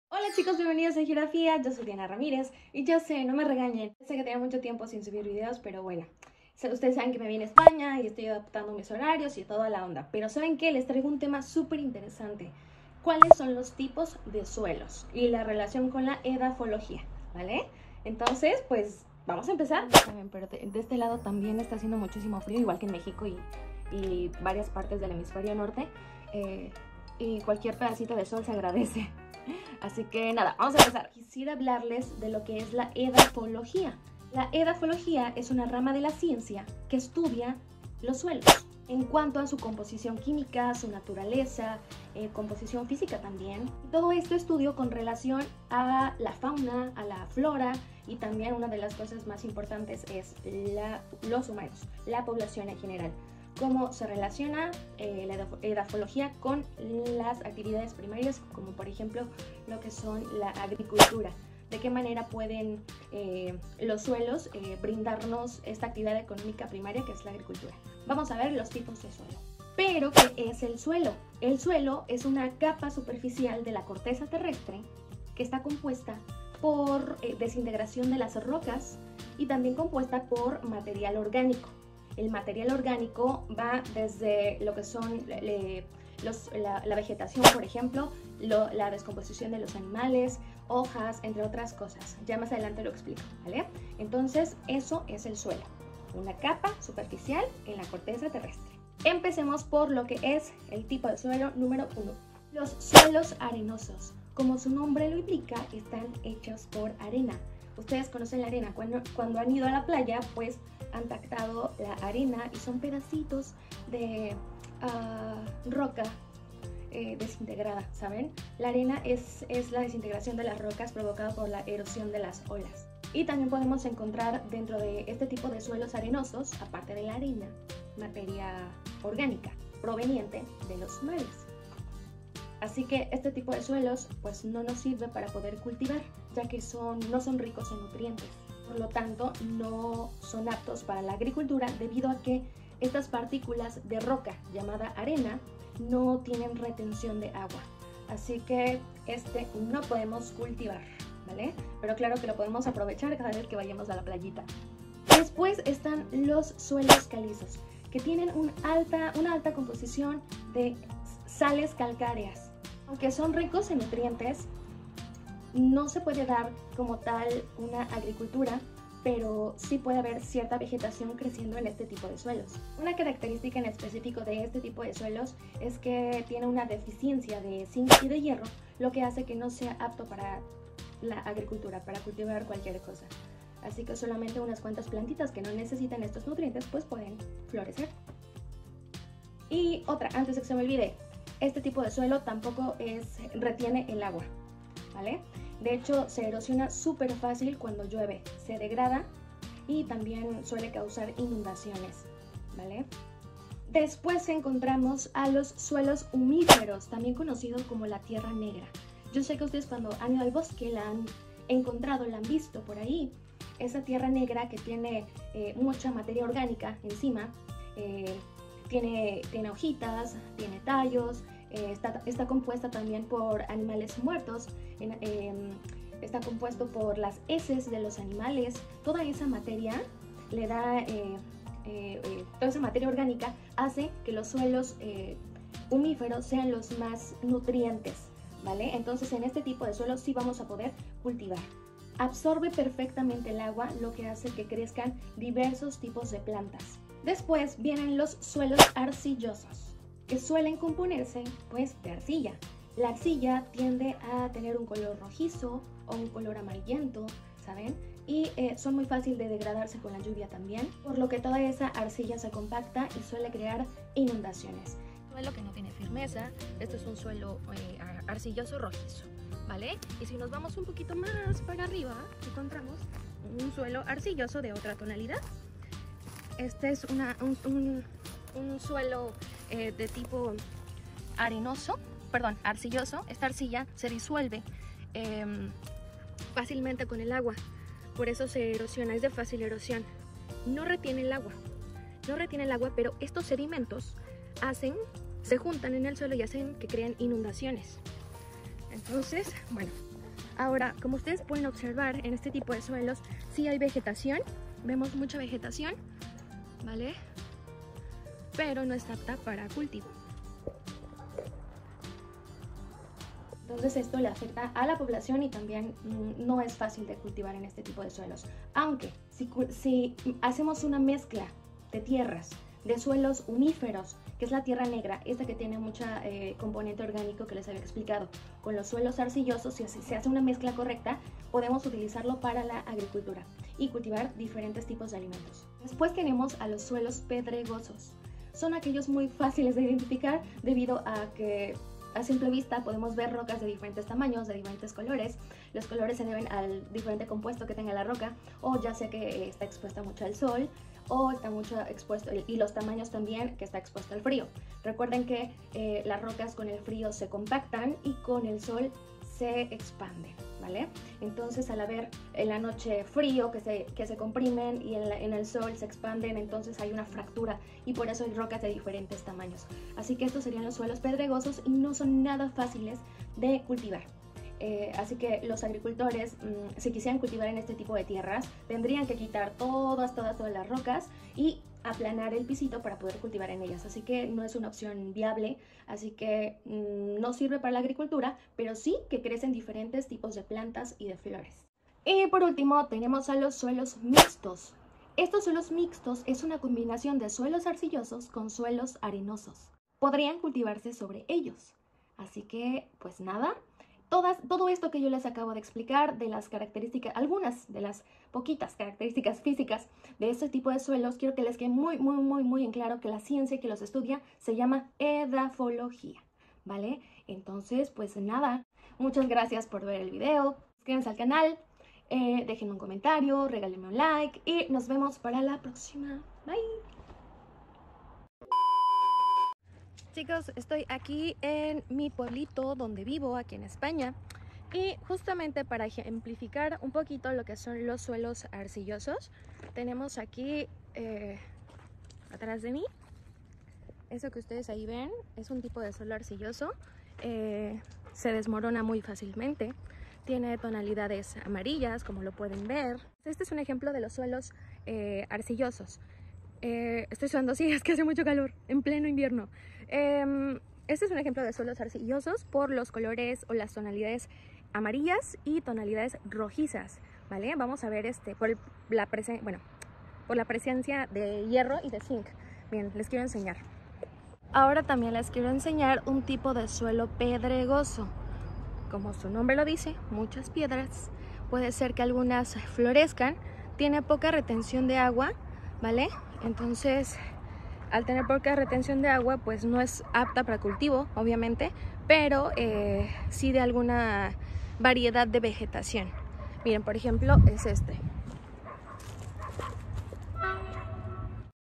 Tipos de suelo/ Edafología.🍂
Hola qué tal chicos, bienvenidos a geografía yo soy Diana Ramírez y el día de hoy les traigo un tema interesante relacionado con los tipos de suelo.